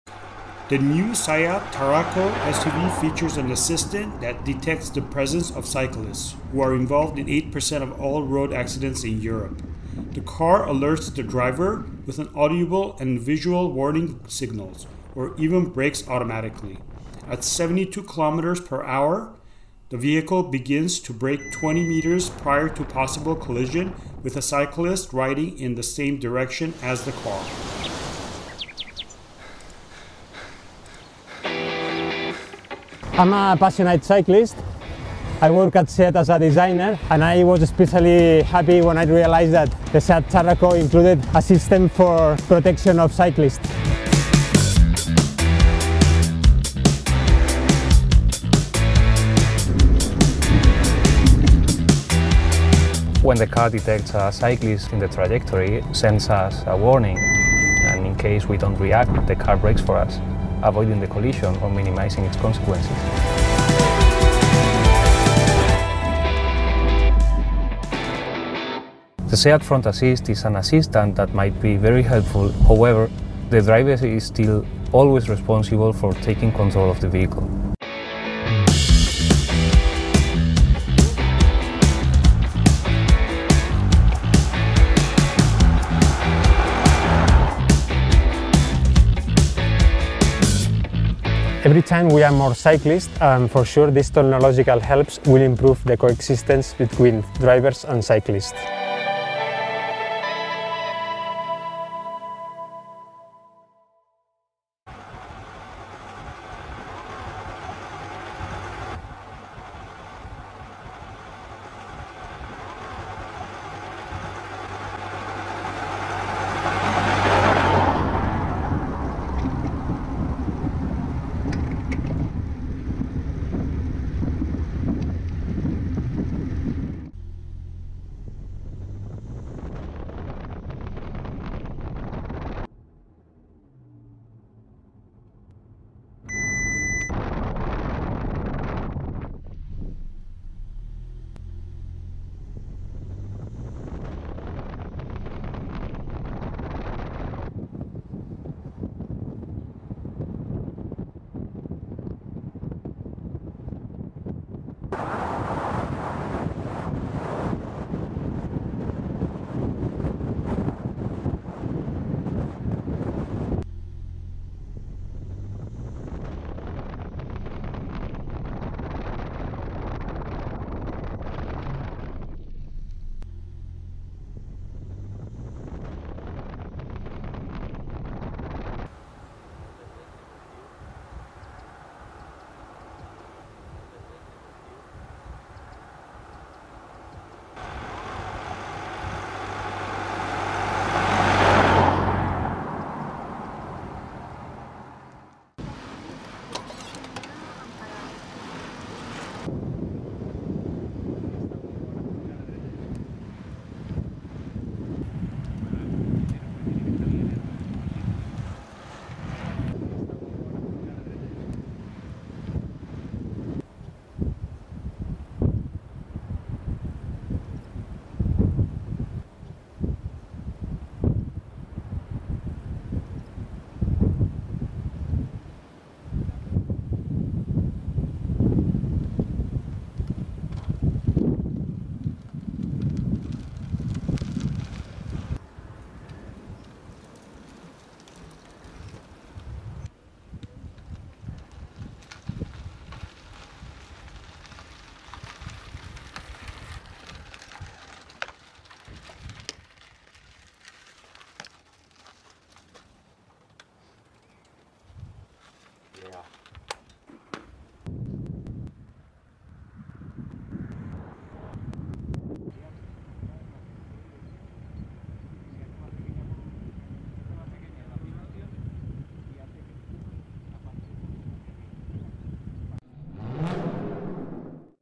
2019 SEAT Tarraco SUV Assistant System
A SEAT engineer and two of his cycling enthusiast friends take to the road to show us how the new driving assistant on the SEAT Tarraco operates to protect riders, as they are involved in 8% of traffic accidents in Europe. This is how it works:

-A car with the ability to recognise a bicycle: At the wheel of the SEAT Tarraco, Esteban Alcantara, who is responsible for Active Safety at SEAT, encounters a couple of cyclists travelling in the same direction. When he gets closer to them, "the front mounted elradar detects their presence, and according to the trajectory and speed of both the car and the cyclists, triggers a number of actions to prevent a possible collision", he says.

-Every second counts: When the assistant on the SEAT Tarraco detects an imminent collision in 1.5 or 2 seconds, it activates an audible  and visual warning. If the driver does not react, the car automatically begins an emergency braking manoeuvre between 0.8 and 1 second before the possible  accident takes place. According to this engineer, in a real driving situation this means that "when driving down a road at 72 km/h, the car would begin to respond approximately 20 metres before a possible collision", he adds.

- 1,200 development tests: This is how many trials this assistant goes through, by recreating 450 different  scenarios on a track. "Simulations are performed in severalreal driving conditions. Structures are used that reproduce the properties of different road users, such as pedestrians or cyclists, to test how the vehicle responds in each situation", explains this engineer.

-The driver is ultimately responsible: Alcantara acknowledges  that driving  assistants  are of great help at the wheel: "Active Safety systems play an increasingly important role in protecting road users, and in some cases can prevent accidents or minimise their consequences." However, this expert points out that the person driving the car is always ultimately responsible. "This kind of assistant is no substitute for the driver's obligation to remain alert, respect traffic regulations, reduce speed and maintain the safety distance when passing a cyclist", he states. At the same time, cyclists have to ride on the right, wear a bicycle helmet in built-up  areas, and they are not allowed to ride in a group.

- Keeping everyone safe: "At the SEAT R&D Centre we are sensitive about protecting drivers as well as all other road users", says Esteban. Furthermore, company designer and amateur cyclist Tony Gallardo also emphasises that "more and more people are taking a liking to this sport, so we must stay committed to promoting the correct conduct on the road and to keeping everyone safe", he adds.